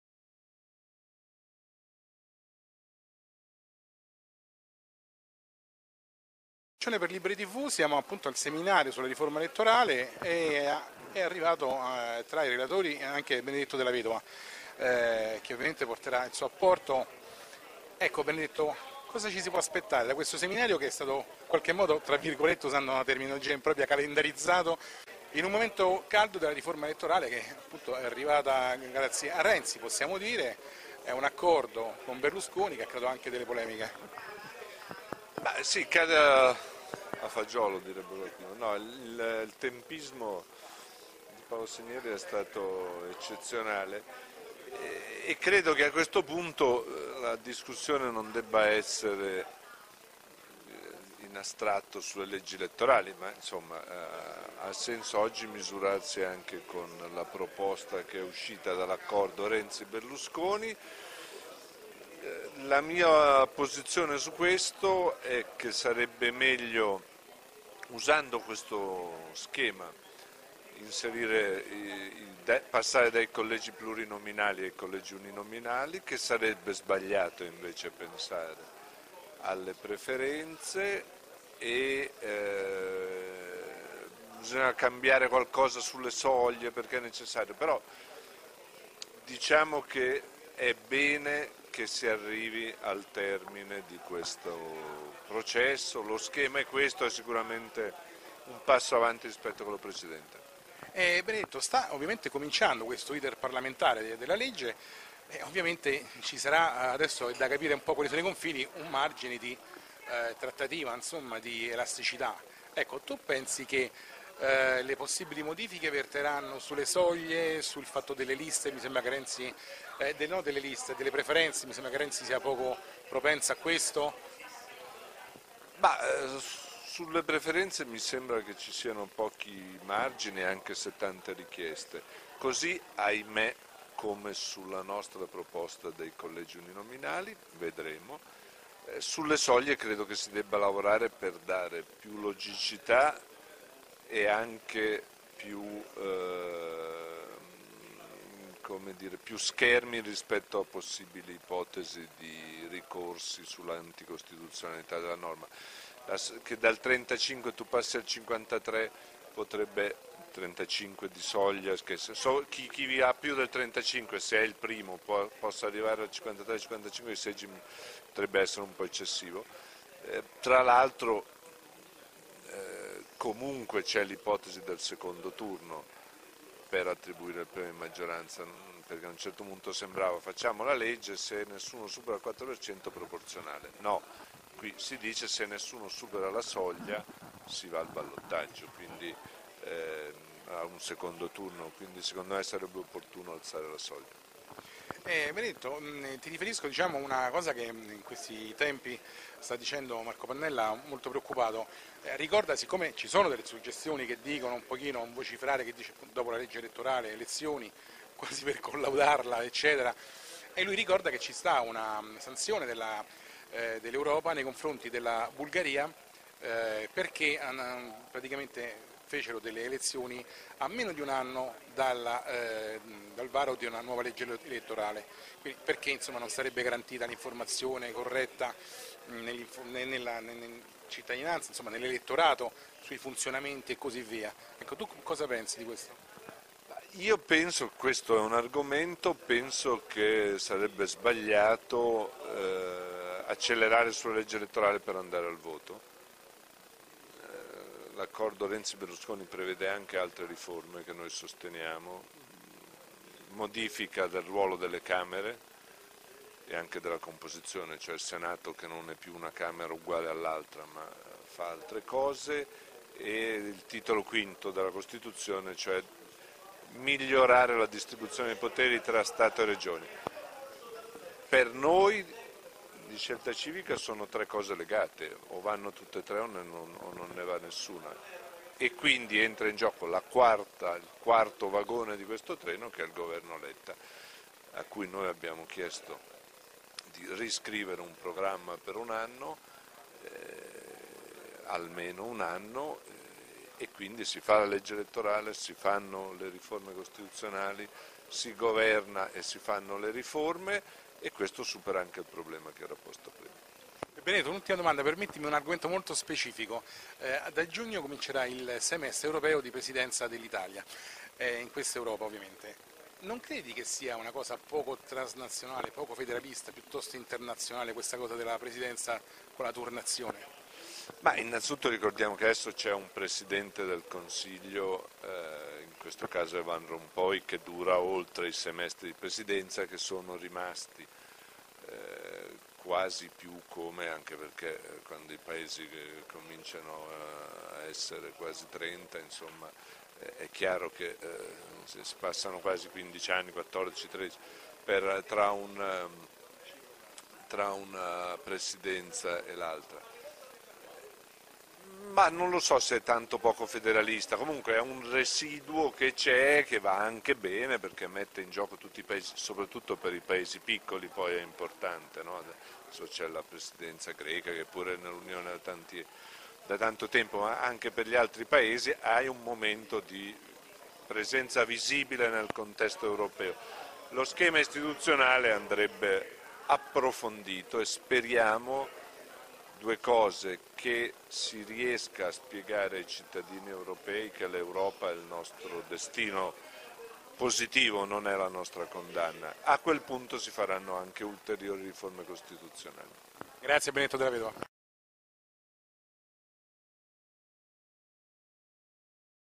La Riforma della legge elettorale: Intervista a Benedetto Della Vedova (Scelta Civica)
Registrazione effettuata da liberi.tv a Roma sabato 25 gennaio 2014 presso l'Hotel Aleph Boscolo.
Seminario sulla Riforma della legge elettorale organizzato da "Il Cantiere" -